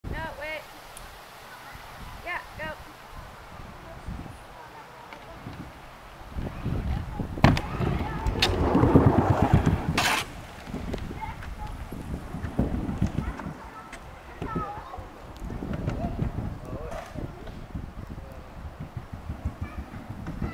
Mt Tremblant Scooter trial on New Pump Track
Joey Cheetham of JFCycles  taking a ride on Mt. Tremblant Pump Track in old town of St. Jovite now a part of Mt Tremblant Region.